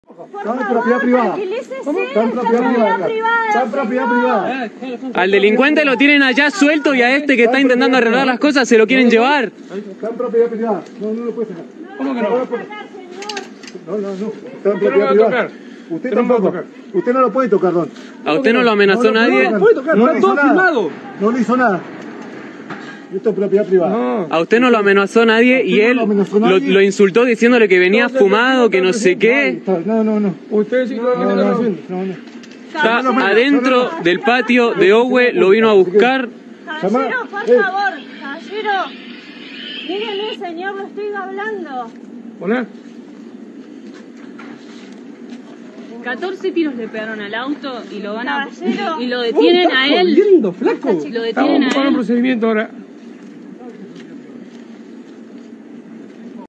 Terror en Neuquén: un policía a los tiros en una cerveceria
El efectivo hizo al menos 9 disparos. Los testigos denunciaron además "abuso policial", ya que el dueño de la cervecería fue detenido de forma injusta y violenta.